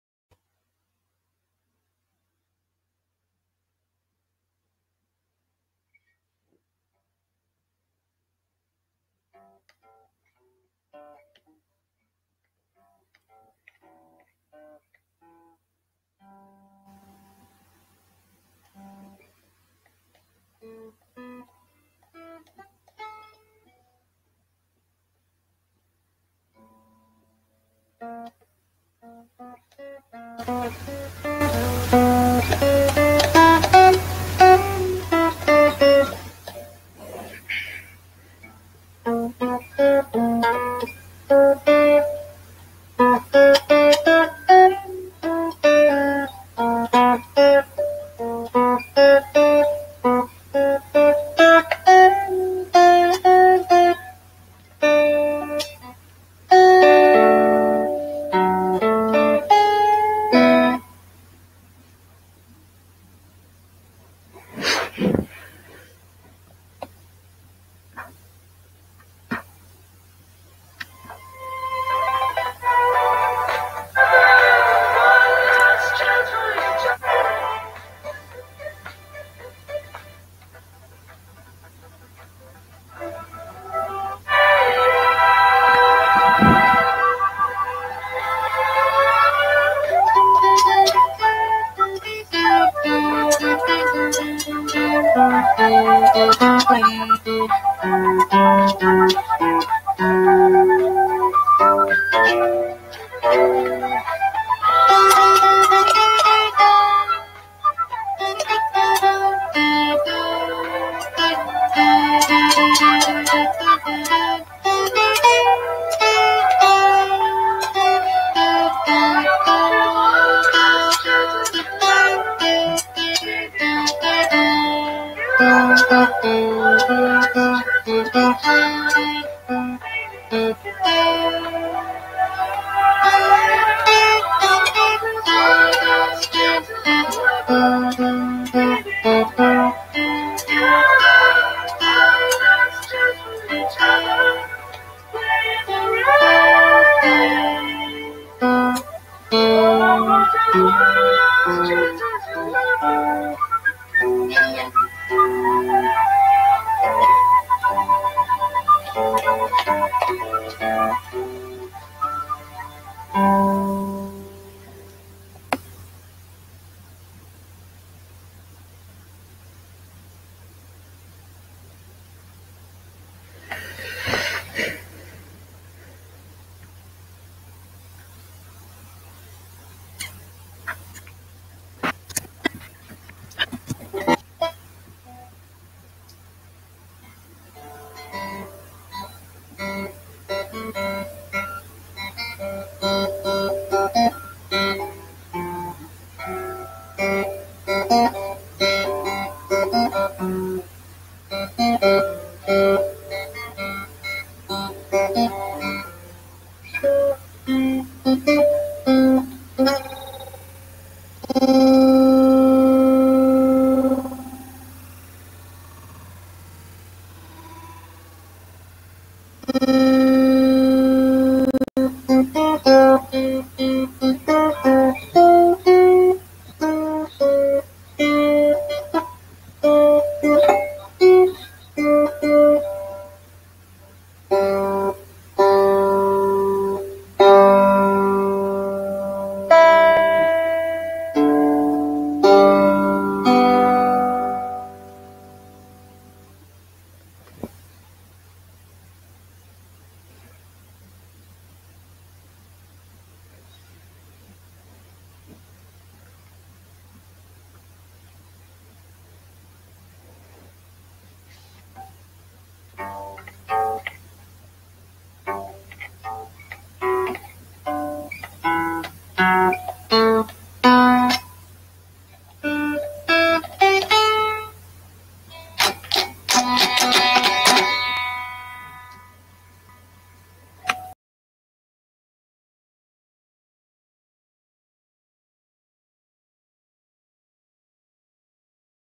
guitar noodling at far too late an hour
this was recorded a little while ago, i just didnt get around to editing it